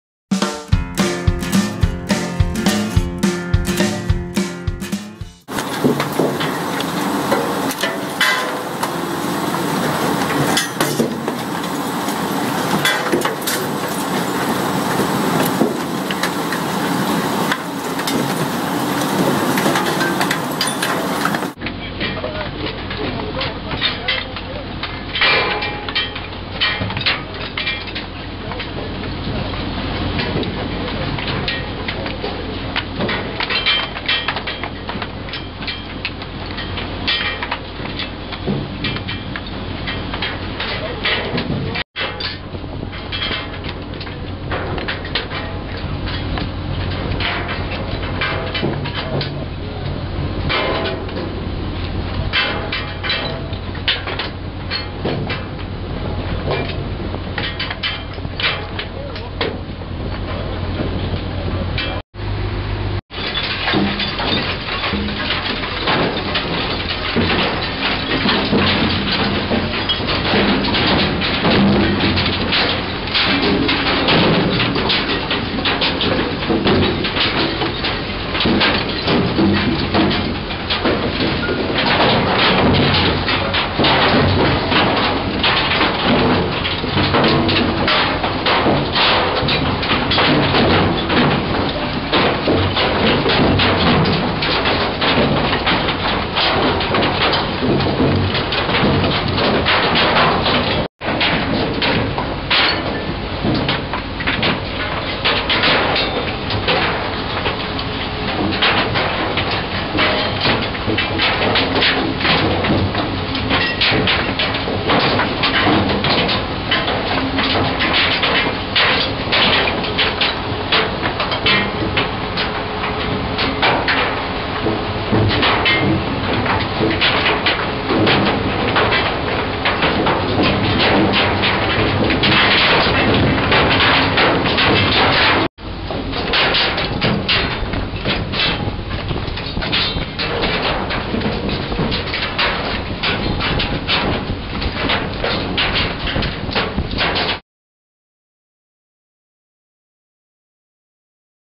Industriavfall Holland
Sortering av industriavfall där man avskiljer papper, plast, sten och betong från övrigt brännbart material.